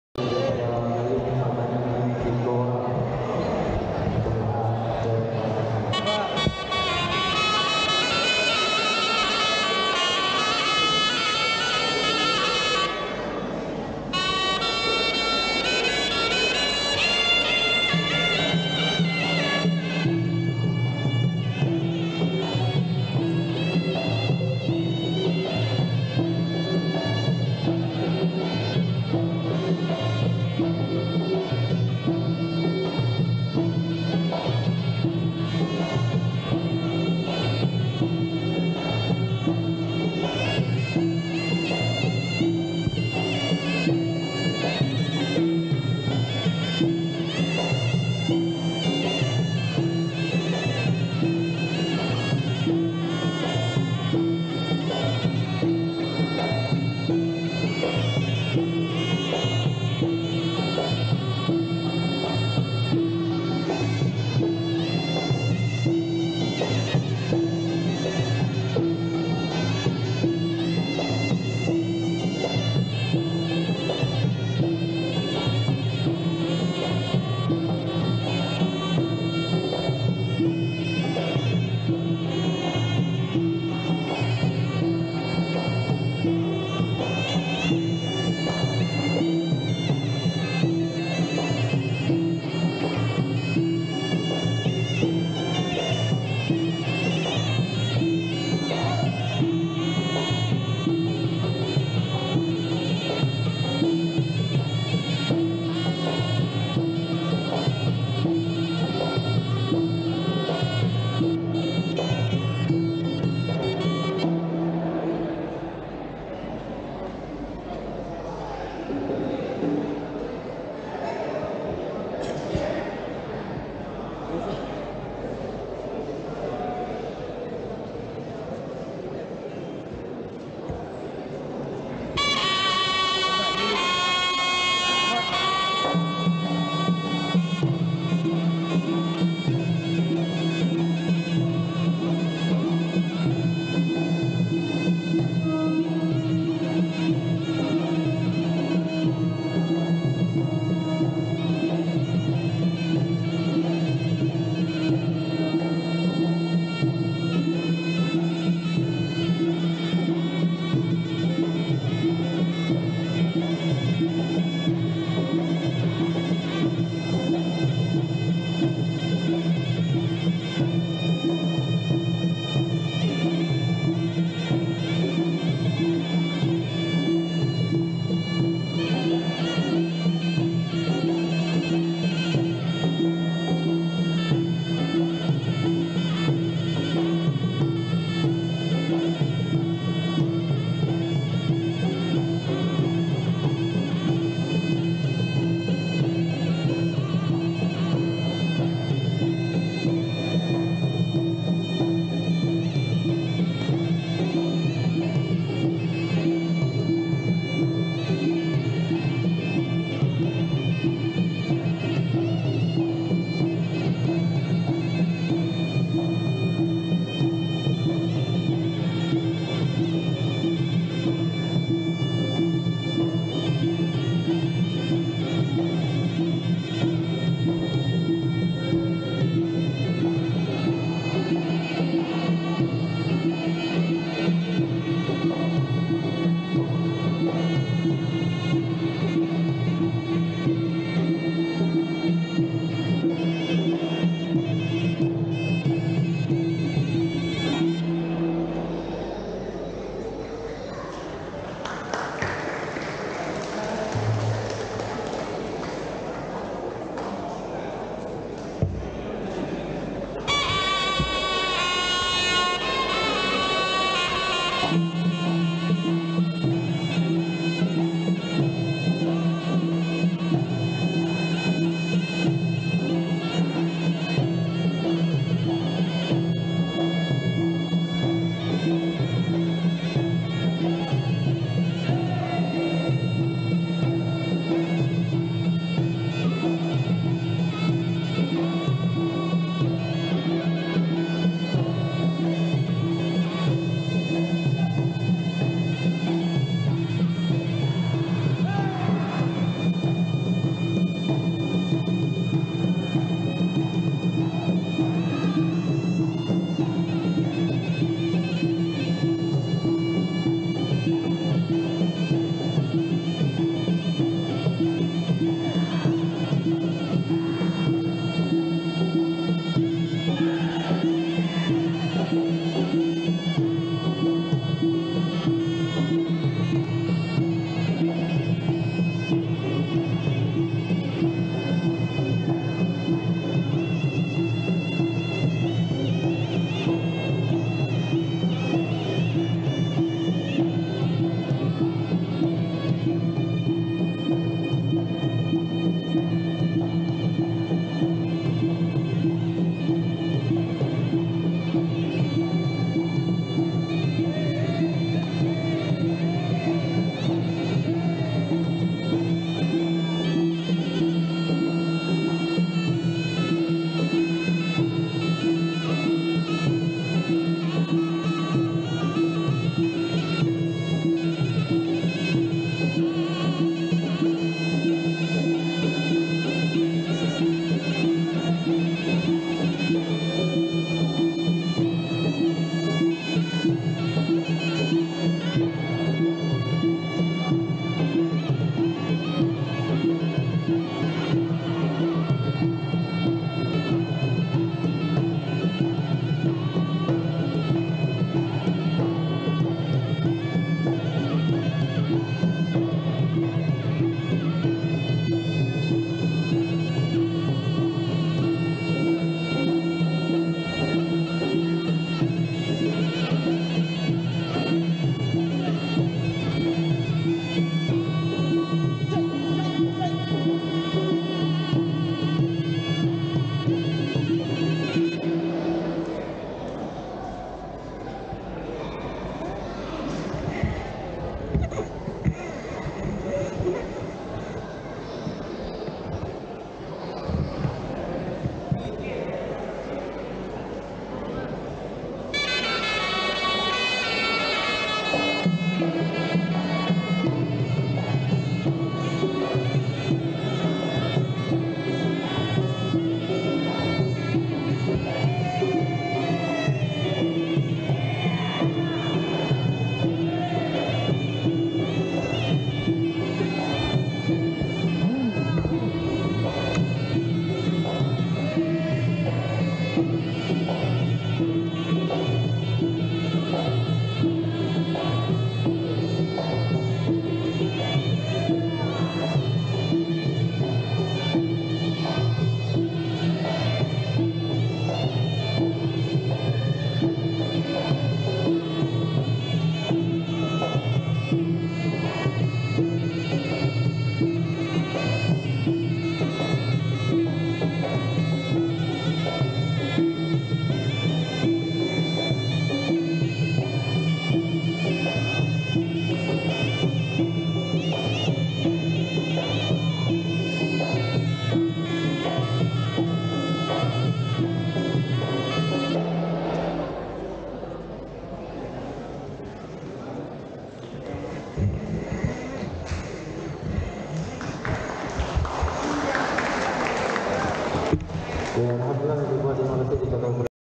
Silat - Persembahan 6
PERSEMBAHAN SENI SILAT WARISAN BUDAYA SALASILAH KELUARGA MELAYU PULAU PINANG 2023